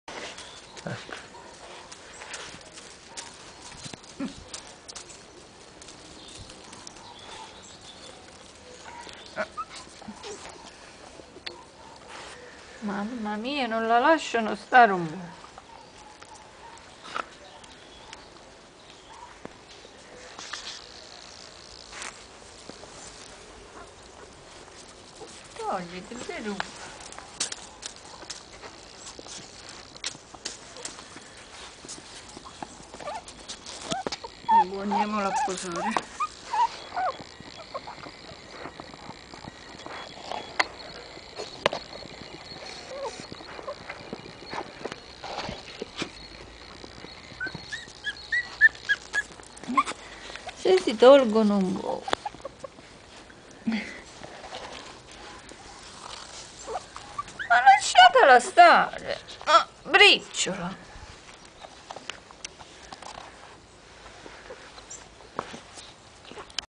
piccolissima cagnolina meticcia
Bellissimo video di una piccolissima cagnolina meticcia. Con lei ci sono la madre (Nocciolina di colore marrone), la zia (Briciola di colore nero) e il cugino più grande (Beluga di colore bianco).